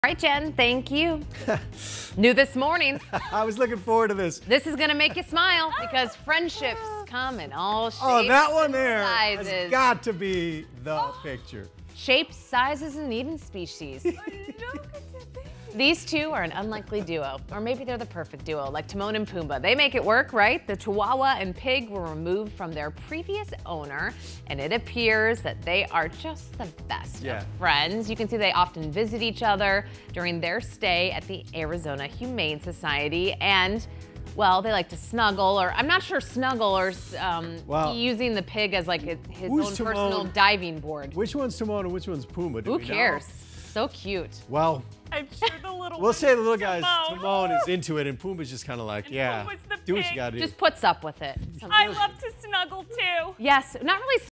Chihuahua and pig at Arizona Humane Society are the best of friends
Similar to Timon and Pumbaa from the movie "The Lion King," this tiny chihuahua and plump pig from the Arizona Humane Society are the cutest besties.